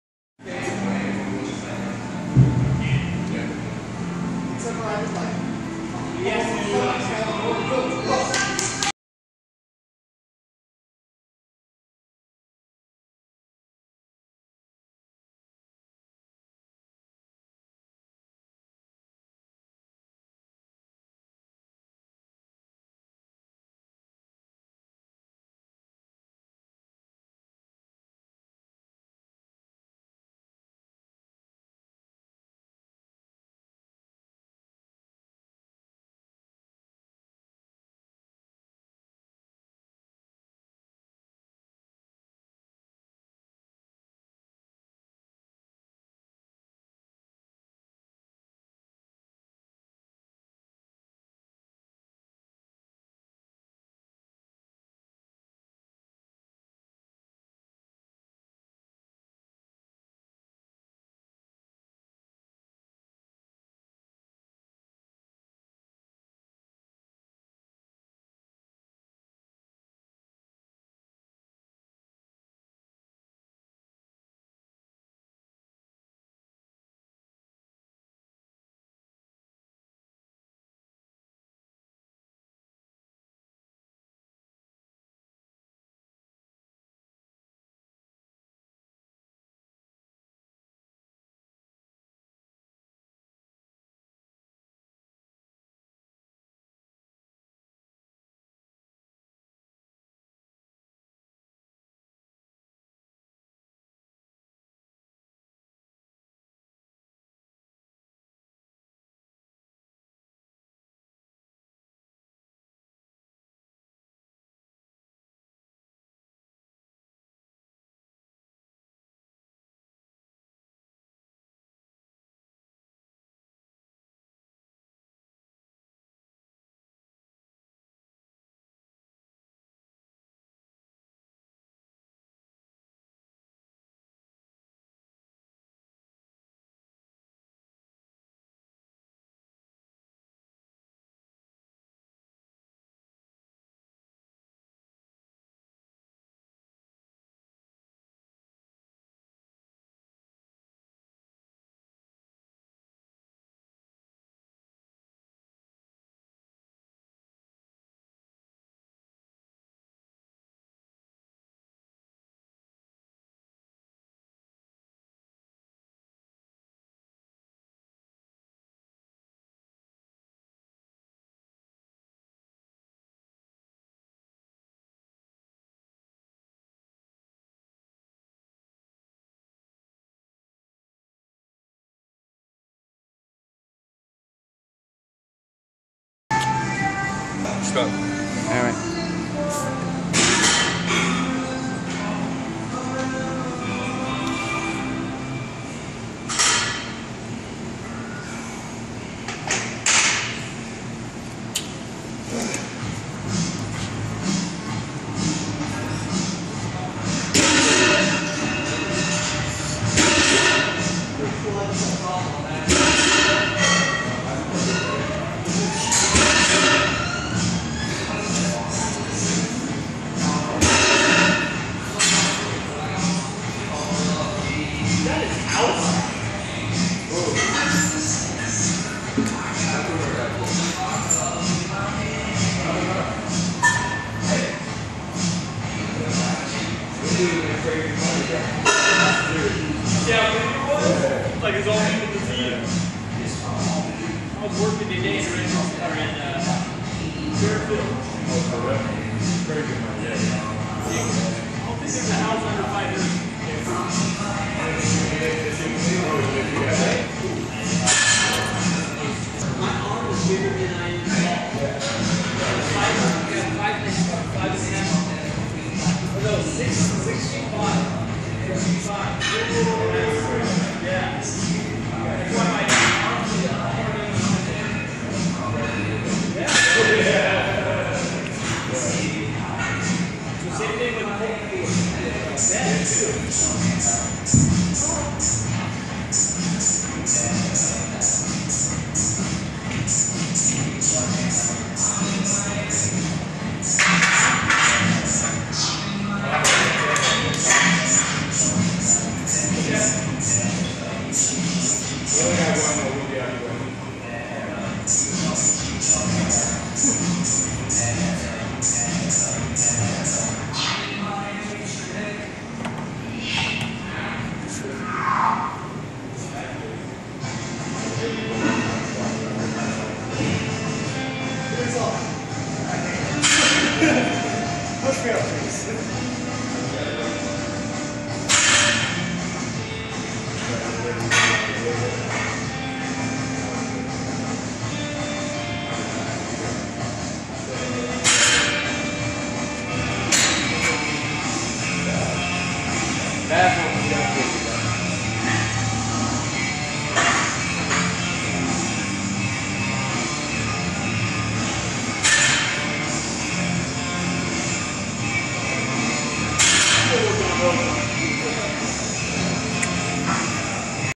Andreas Cahling - light weight shoulder raises
Light weight high repetition dumbbell shoulder raises. Off season training at World Gym in Vista.